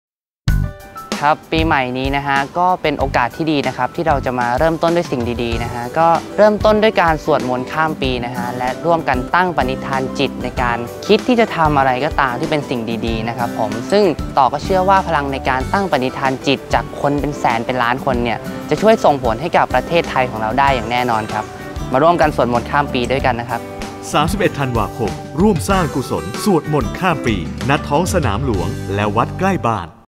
สวดมนต์ข้ามปี ต่อ ฮอร์โมน
ครับ ปีใหม่นี้นะครับ ก็เป็นโอกาสที่ดีนะครับ ที่เราจะมาเริ่มต้นด้วยสิ่งดีๆ เริ่มต้นด้วยการสวดมนต์ข้ามปีนะครับ และร่วมกันตั้งปณิทานจิตในการคิดที่จะทำอะไรก็ตามที่เป็นสิ่งดีๆนะครับผม ซึ่งต่อก็เชื่อว่าพลังในการตั้งปณิทานจิต จากคนเป็นแสนเป็นล้านคน จะช่วยส่งผลให้ประเทไทยของเราได้อย่างแน่นอนครับ มาร่วมกันสวดมนต์ข้ามปีด้วยกันนะครับ 31 ธันวาคม ร่วมสร้างกุศลสวดมนต์ข้ามปี ณ ท้องสนามหลวงและวัดใกล้บ้าน

และขอเชิญชวนคนไทยมาร่วมส่งคำตั้งมั่นของตนเองที่จะทำสิ่งดีๆ ตลอดปีหน้าเพื่อสร้างองค์ "พระทำ" พระพุทธรูปที่ถูกสร้างจากการตั้งปณิธานจิตลงบน Post-it กว่า 100,000 ใบ ผ่าน